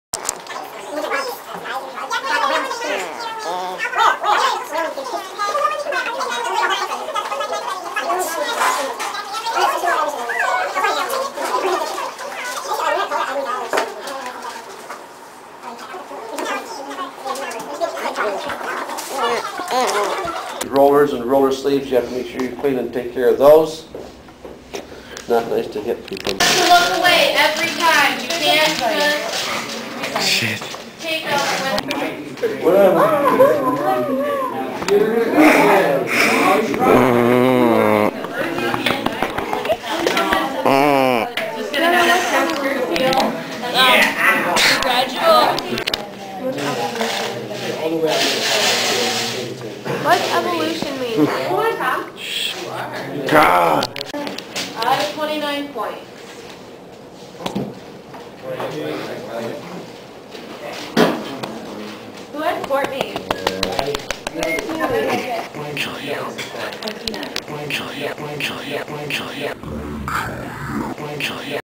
Sleeper
Scott sleeps in class.. and i wake him in class. Wooh!